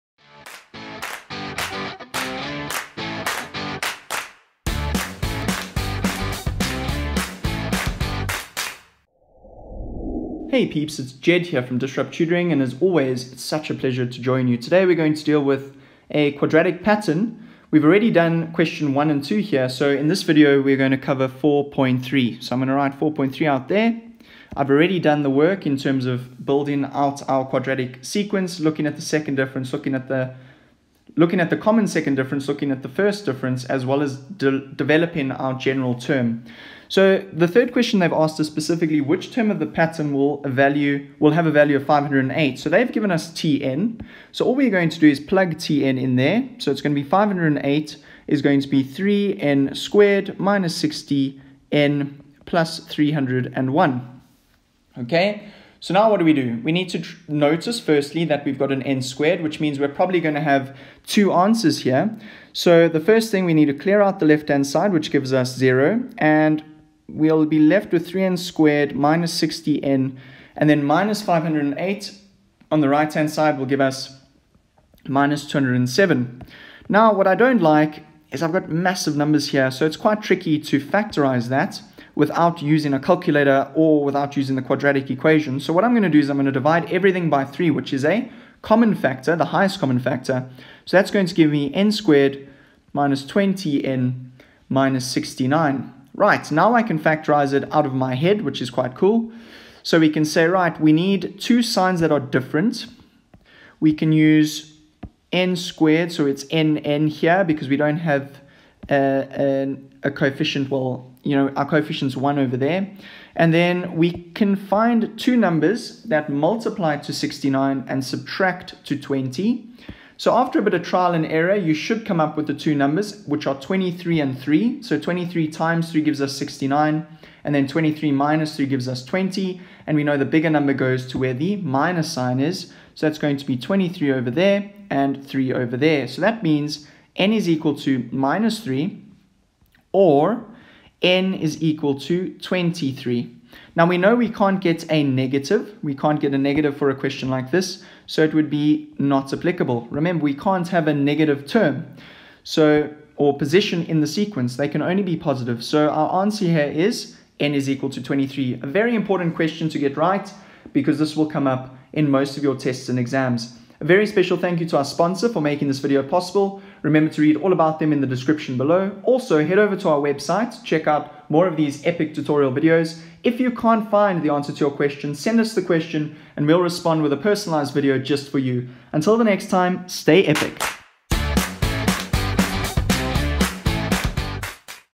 #358 Finding the position of a term in a quadratic pattern
This question was expertly tutored by Jedd Harris.

The question covered in this tutorial video was sourced from a past paper (DBE November 2019).